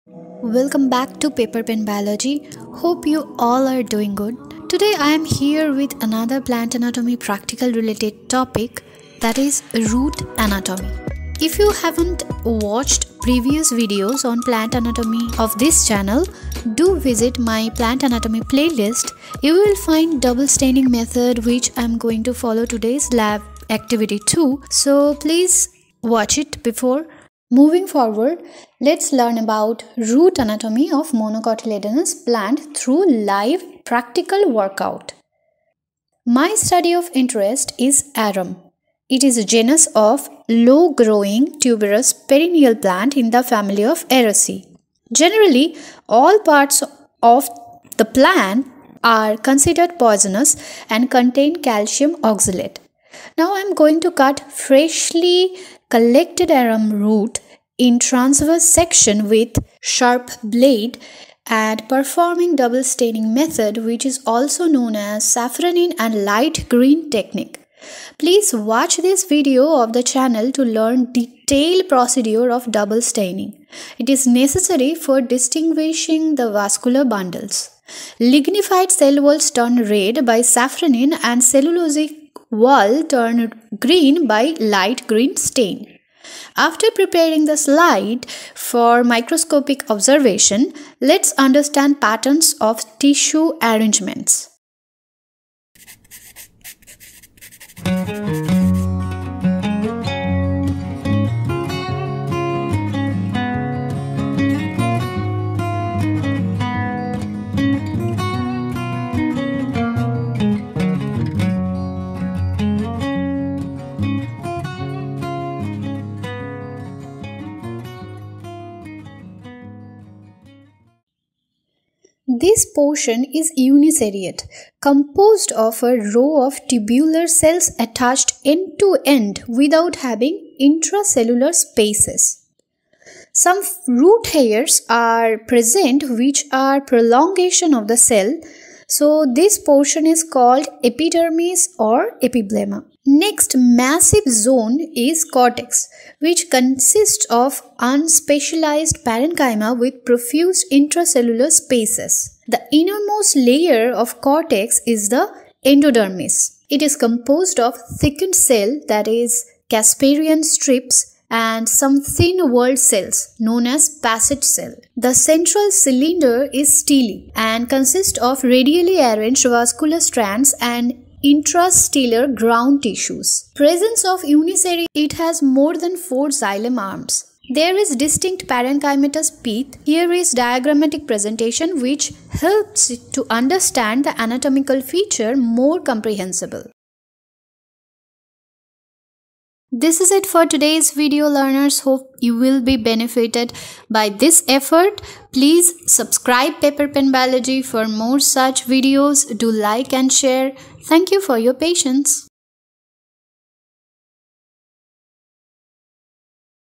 Monocot root anatomy | T.S of monocot root | Monocot root diagram
Transverse section of monocot root is cut in live work out,followed by double staining method.
In this video---
0:00 Intro
0:28 T.S of Arum sp. root
1:36 Monocot root diargram drawing
3:08 Outro

SUGGEST us for betterment of our channel.
BOOKS CREDIT-College botany(S.C Santra,T.P Chatterjee,A.P Das
IMAGE CREDIT-Paperpenbiology
VIDEOS You may find useful in this channel